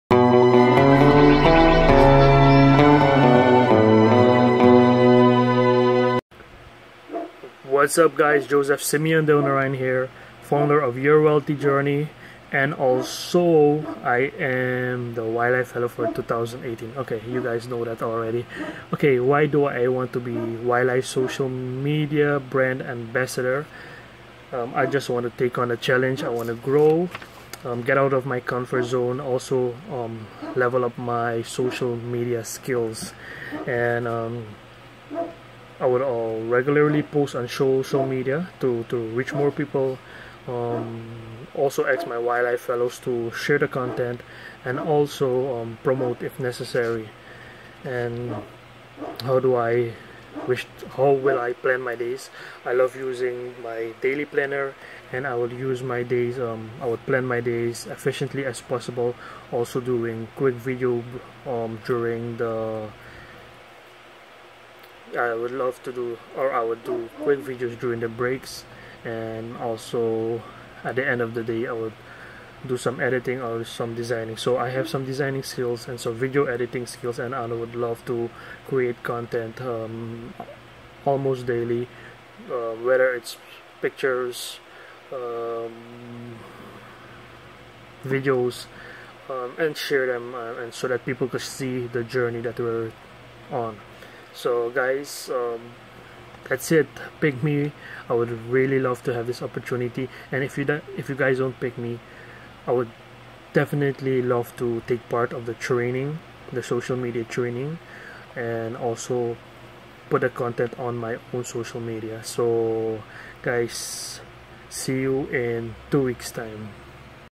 Why Choose Joseph as YLAI Social Media Brand Ambassador for DENVER, Colorado?
In this short video, you will find the reasons why I've applied for the role as YLAI Social Media Brand Ambasador in Denver.